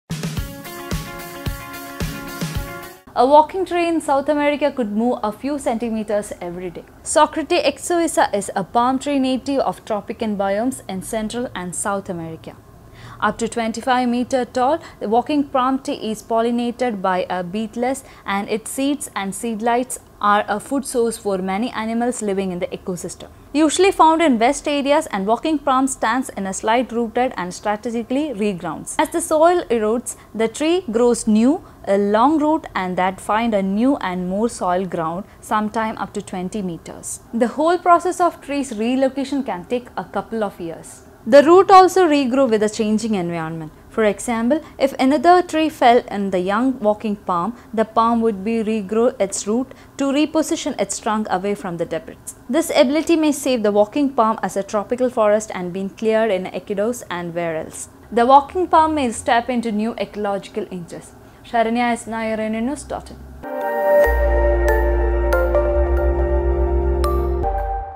The walking tree of South America
The Walking Palm may step into new ecological niches.

Socratea exorrhiza is a palm tree native to tropical biomes in Central and South America.  Up to 25 meters tall, the Walking Palm is pollinated by beetles and its seeds and seedlings are a food source for many animals living in this ecosystem.  Usually found in wet areas, the Walking Palm stands on stilted roots that strategically regrow. “As the soil erodes, the tree grows new, long roots that find new and more solid ground, sometimes up to 20m.